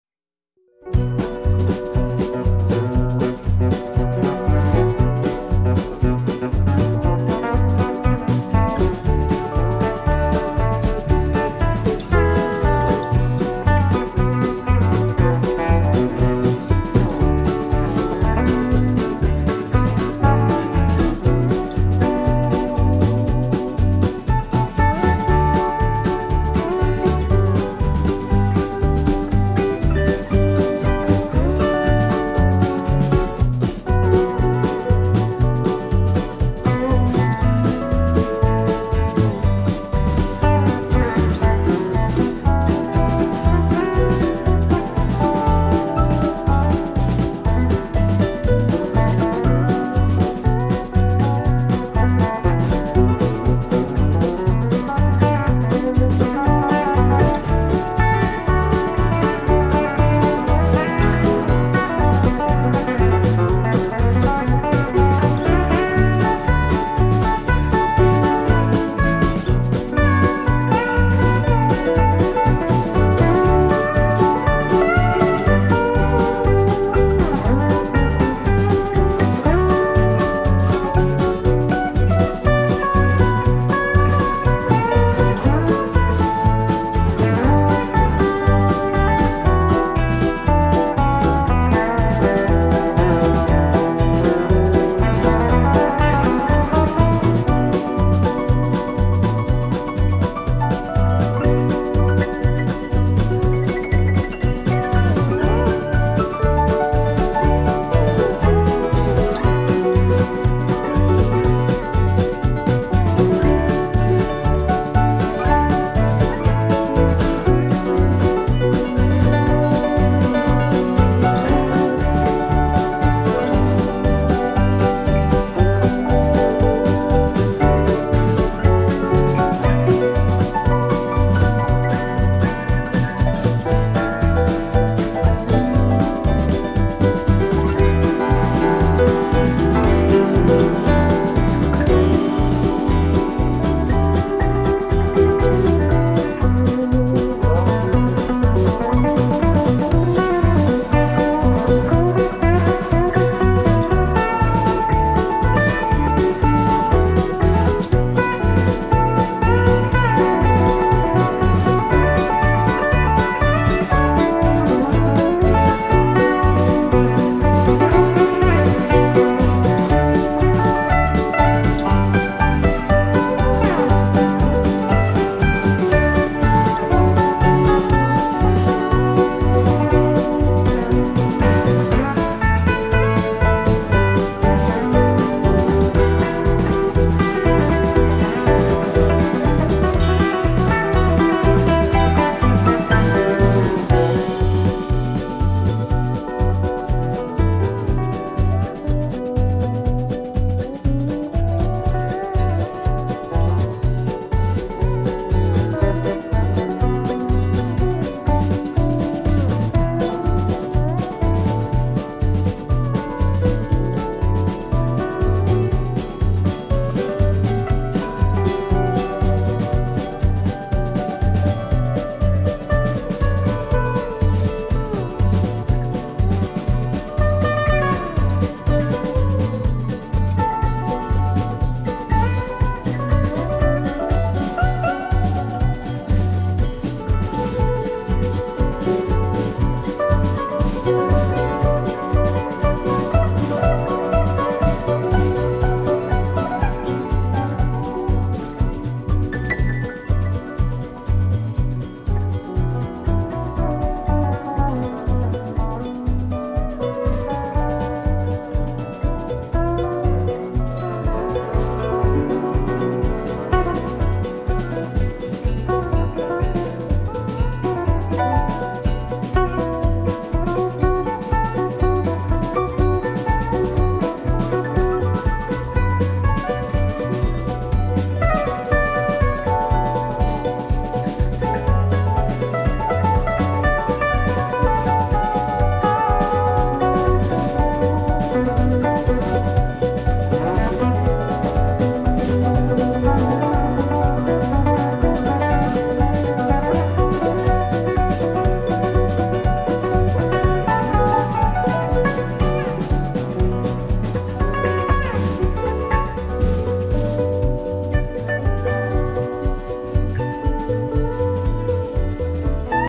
handcuffed and shackled, over the railroad tracks / schwer gefesselt auf dem Bahndamm - Bunikon
Submissive, handcuffed and shackled, walks in the snow over the railroad tracks on the embankment

Sexy Frau, mit Handschellen und Fusschellen gefesselt, läuft im Schnee über die Eisenbahnschienen auf dem Bahndamm